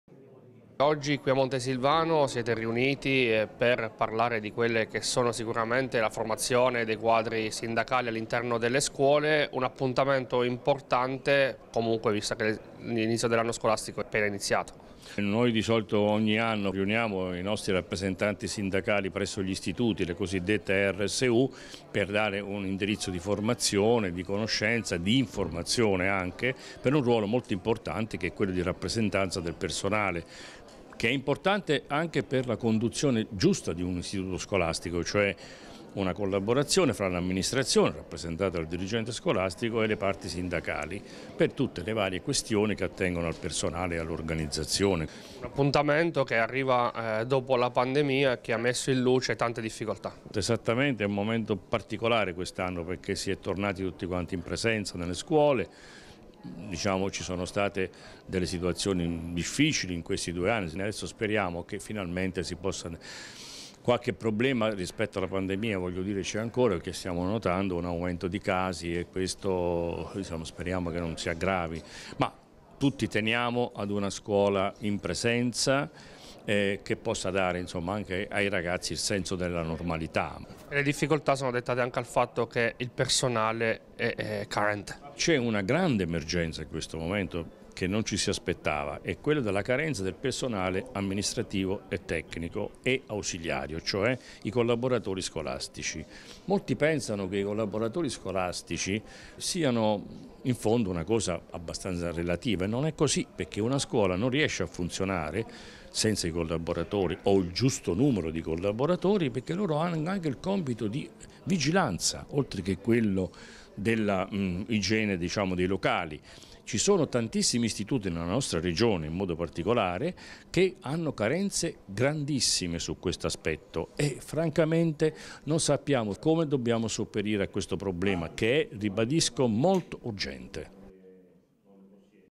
Scuola. Snals Abruzzo forma i quadri sindacali
La funzione dei rappresentanti sindacali all’interno delle scuole. È uno dei temi affrontati dallo Snals Abruzzo. Tra gli argomenti anche la carenza di personale negli istituti.